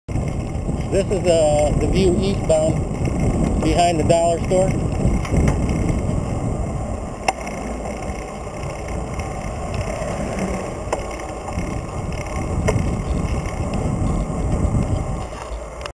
Truck & Trailer in bike lane...
Saw this truck and trailer parked in the bike lane behind the Dollar store on Main St.